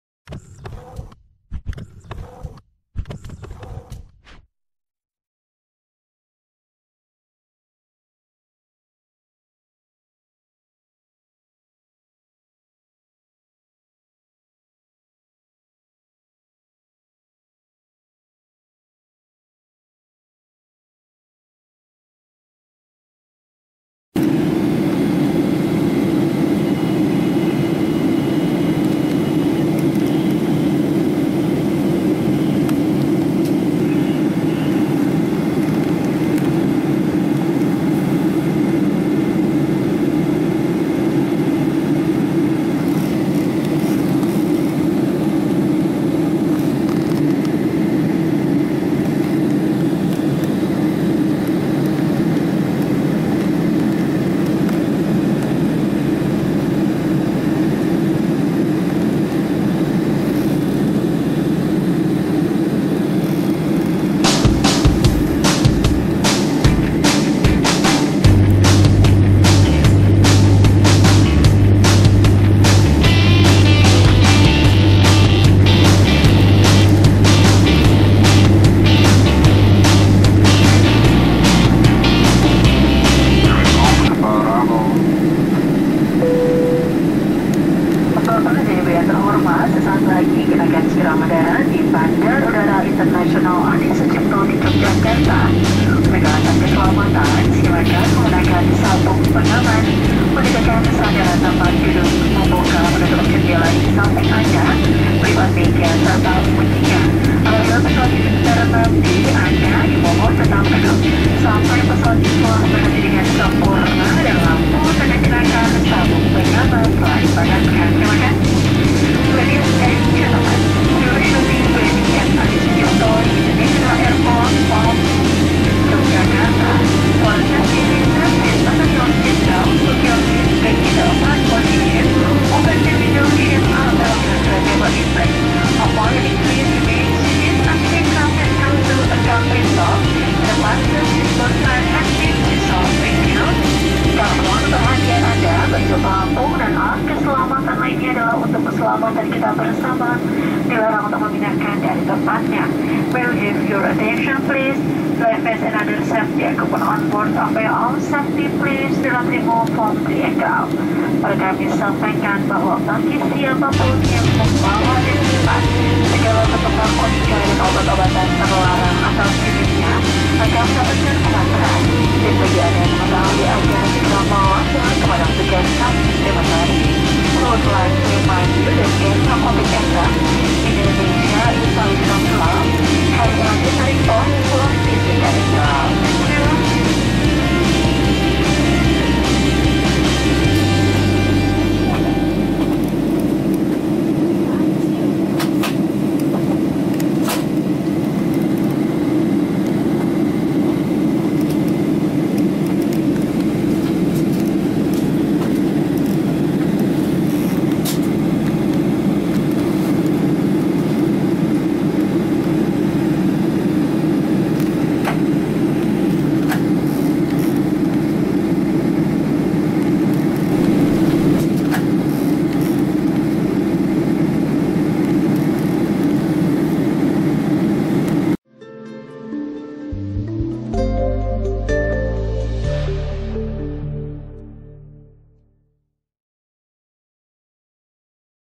SRIWIJAYA AIR Cross Dengan NAM AIR //Gap Ketinggian//saat Cruising Ke Landasan Adi Sucipto Jogja
ini adalah view Dari dalam jendela kabin paling belakang sebelah kanan Pesawat Sriwijaya Air Rute Balikpapan-Jogja Adisucipto, tanggal 30 Oktober 2019. Dimana dalam persiapan untuk melakukan pendaratan Di Bandara Adisucipto terlihat pesawat NAM AIR yang sedang terbang juga, mula mula tampak jauh terbang sejajar namaun semakin dekat berubah arah terbang menyilang hanya saja berbeda ketinggian dan tampak sangat dekat ...tonton videonya

Nyockyee_Explorer Channel